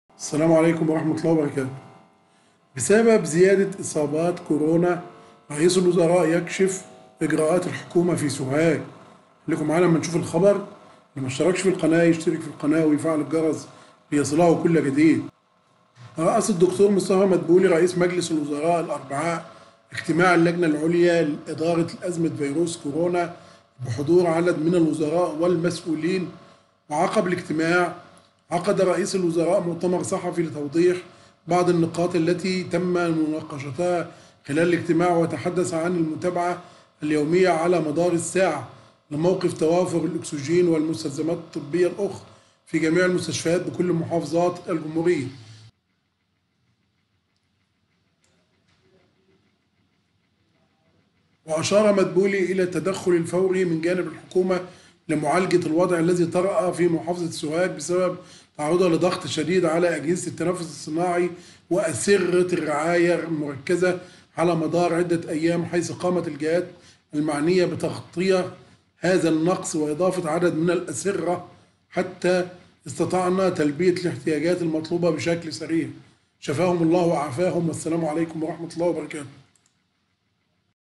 عاجل رئيس الوزراء يكشف اجراءات الحكومة في سوهاج بسبب زيادة اصابات كورونا
نقدم الاخبار المصرية والعالمية